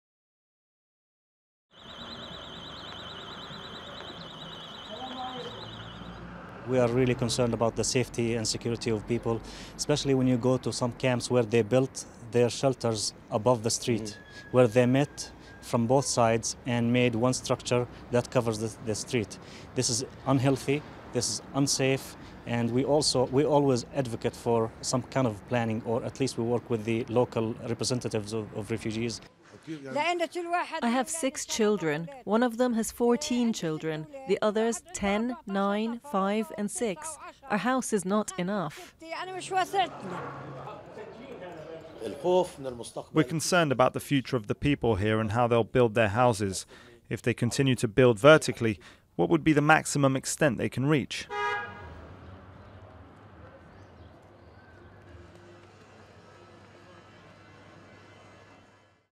With no space to grow, West Bank refugees look upwards
Nael al-Sharif is working on an extension to his property in the Jalazun refugee camp in the Israeli-occupied West Bank. But he is not building outwards -- instead he's expanding upwards. Duration: 00:59